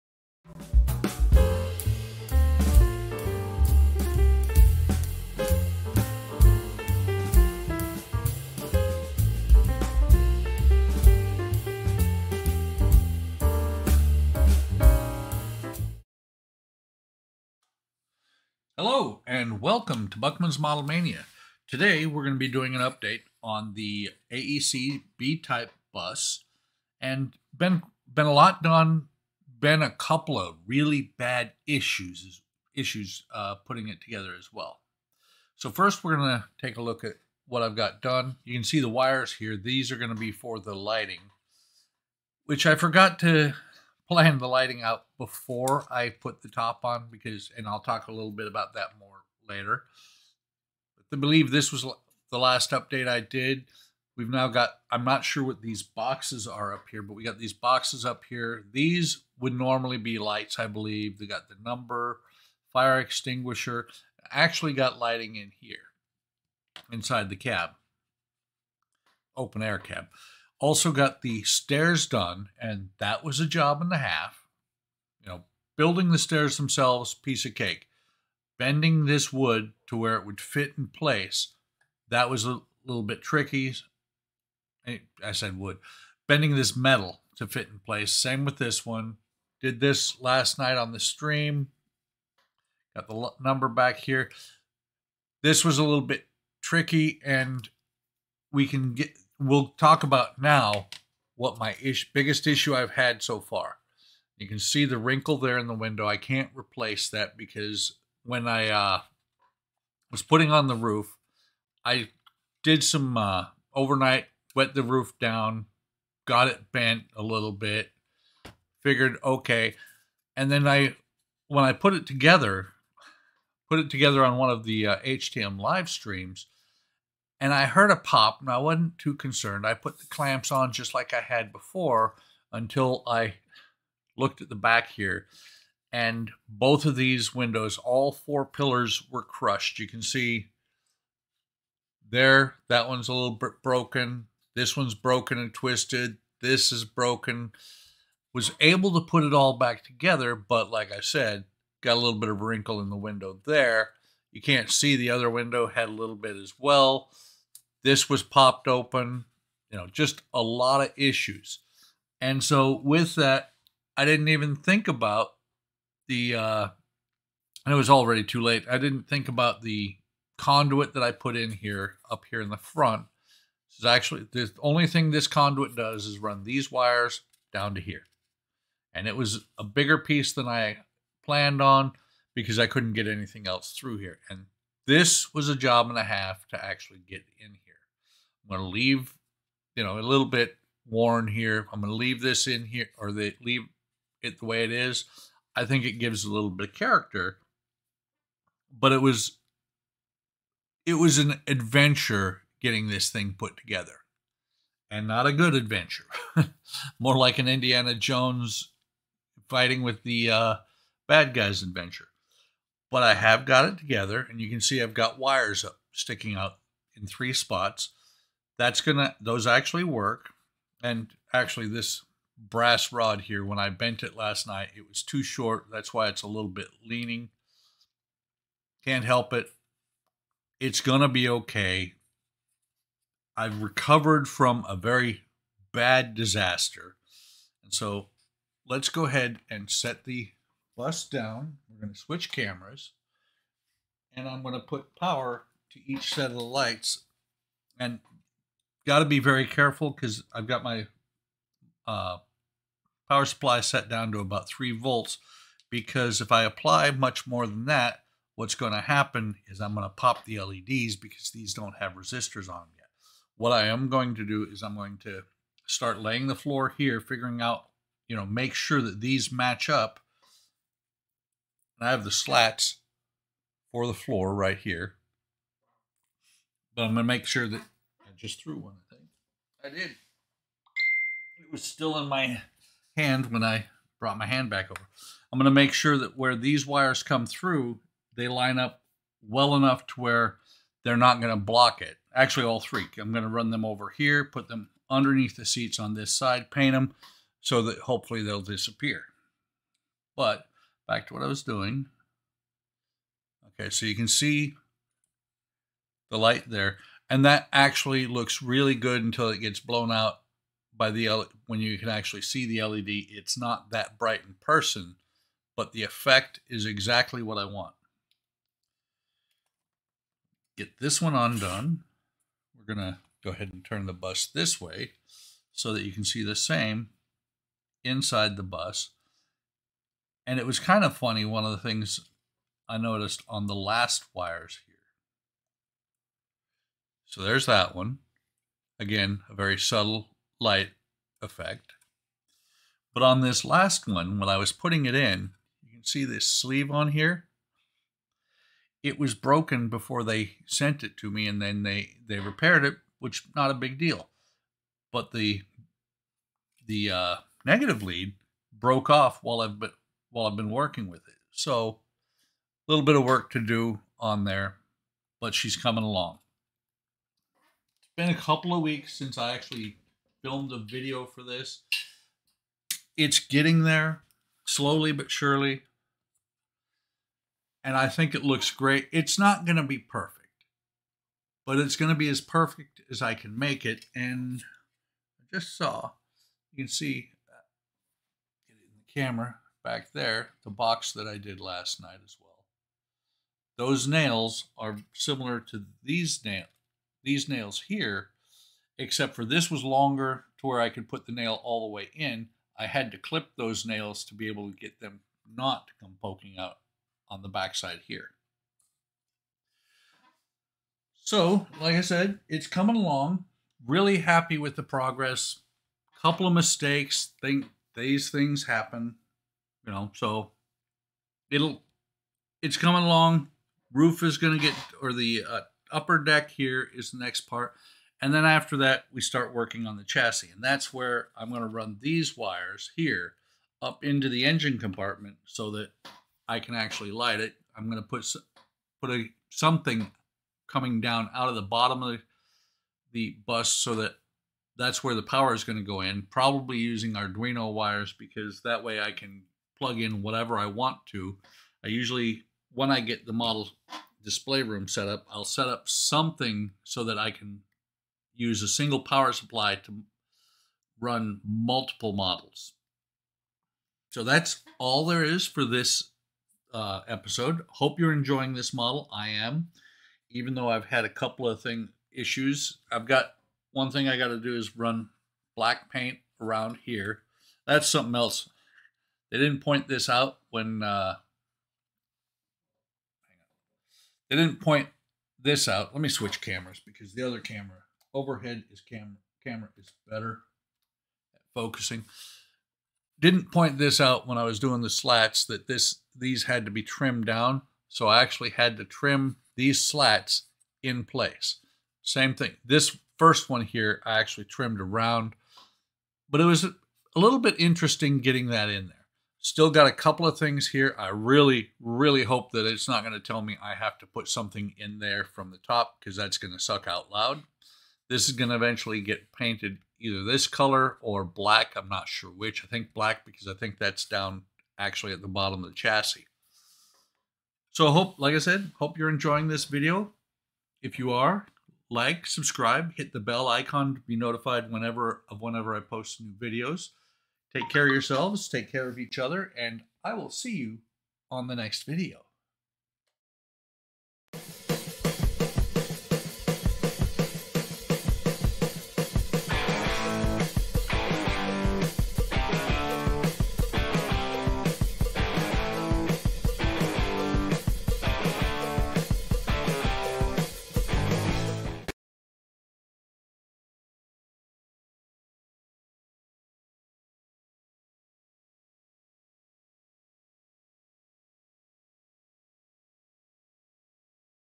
OcCre AEC Bus B Type part 4
Welcome to my build of the 1:24 scale AEC Bus B-Type from OcCre.  B-type buses were built in Walthamstow and replaced the previous X-type buses. The B-type had a seating capacity for 34 persons, 16 inside and 18 on the uncovered top deck and is considered to have been the first mass produced bus. The first of these buses began to carry passengers in 1911. By 1913 around 2500 had entered into service.

Disaster!  While working on the bus on the HTM live stream, I crushed the back of it by clamping the ceiling from the first level/floor of the second level to tight.  However, never give up...I have managed to fix it and I think it will work out just fine.

Thanks for stopping by and I hope you enjoy the show.

And stop by Hobby Time Modelers on Facebook, You Tube and Discord